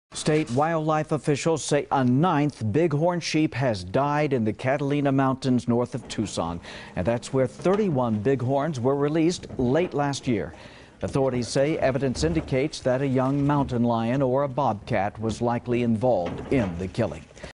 9th bighorn sheep dies in mountain near Tucson
State wildlife officials say a ninth bighorn sheep has died in the Catalina Mountains north of Tucson where 31 bighorns were released late last year.